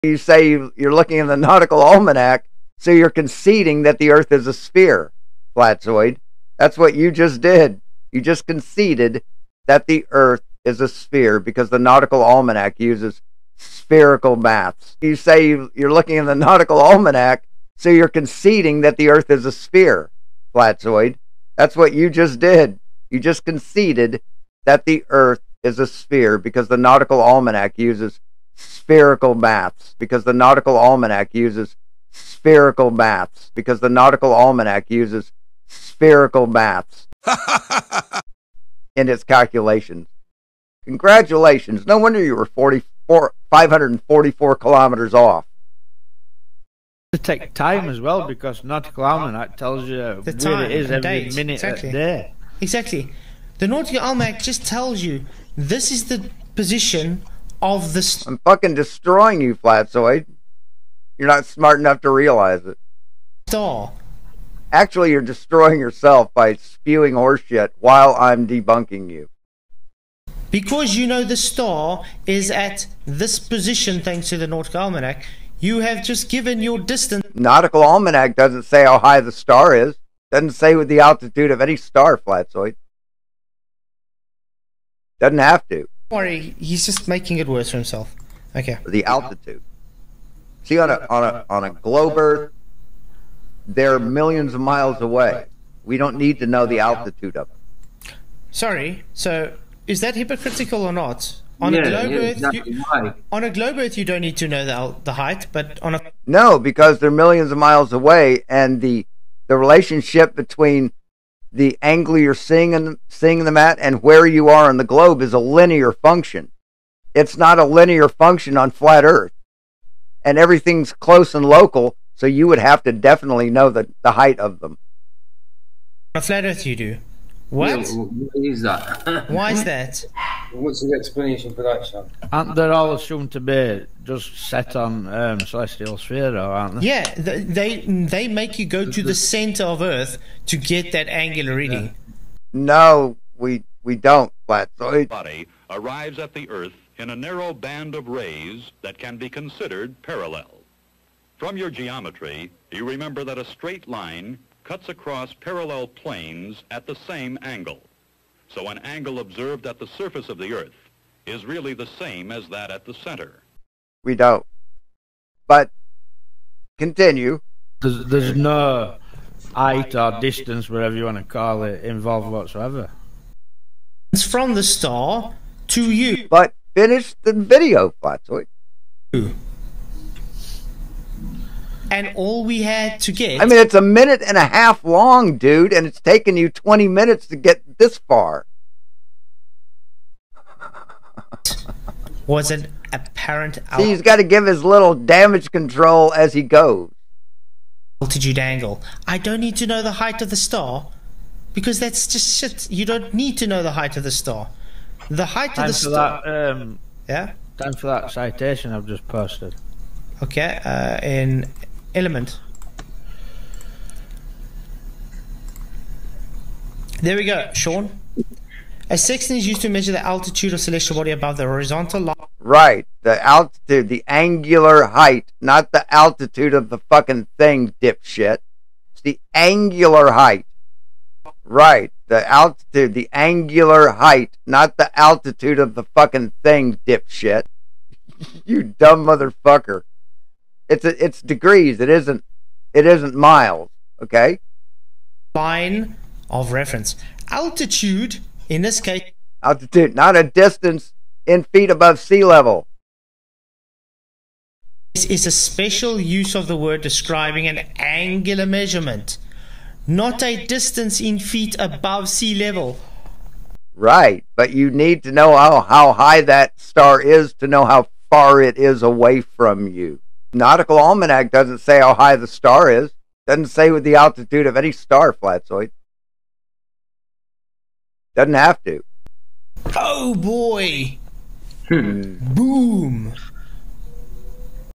Shawn Hawkins debunks himself once again!
@ShawnHawkins666 nice contradictions and double speak you keep showing. Hahaha this is so funny. Tell me again about this so called height?